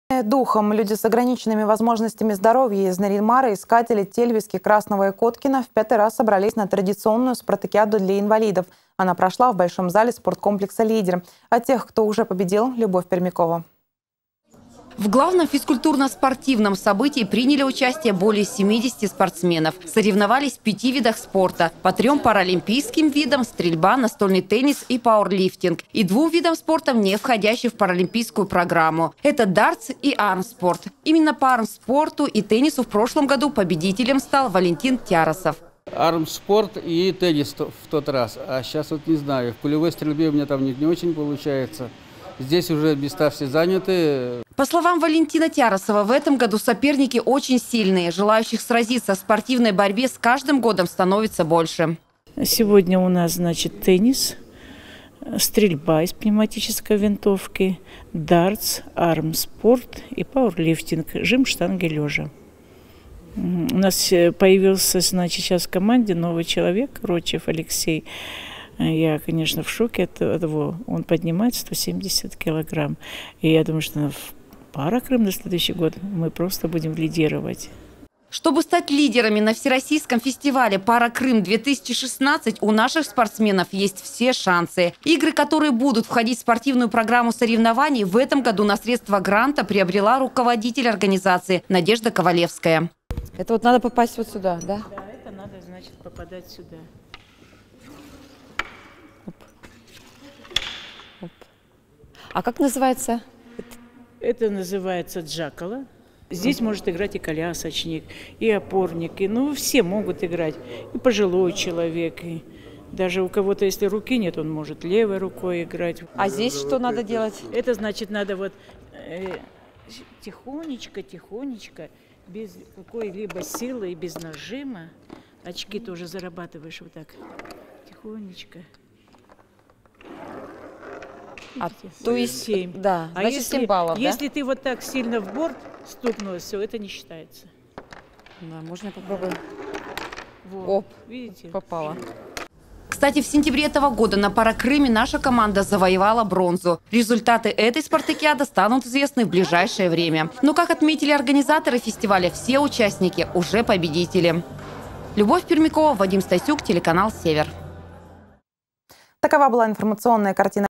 В Нарьян-Маре прошла спартакиада среди людей с ограниченными возможностями здоровья
Сильные духом. Люди с ограниченными возможностями здоровья из Нарьян-Мара, Искателей, Тельвиски, Красного и Коткино в 5 раз собрались на традиционную спартакиаду для инвалидов. Она прошла в большом зале спорткомплекса «Лидер».

В главном физкультурно-спортивном событии приняли участие более 70 спортсменов, соревновались в пяти видах спорта по 3-м паралимпийским видам: стрельба, настольный теннис и пауэрлифтинг, и двум видам спорта не входящим в паралимпийскую программу — это дартс и армспорт. Именно по армспорту и теннису в прошлом году победителем стал Валентин Тярасов.